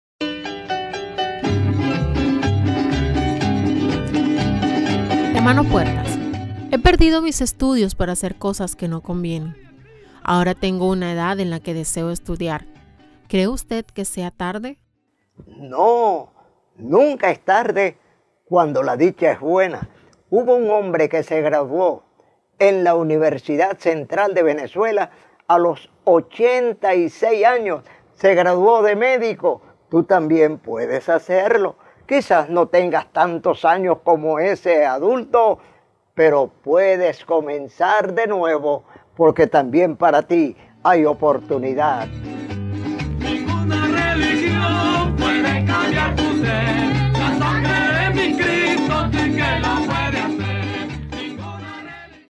HNO PUERTAS RESPONDE #112 | ¿CREE USTED QUE SEA TARDE PARA MÍ ESTUDIAR? | MICRO HNO PUERTAS | LUZ TV
pastor Jaime Puertas:
He perdido mis estudios por hacer cosas que no conviene, ahora tengo una edad en la que deseo estudiar:¿cree usted que sea tarde?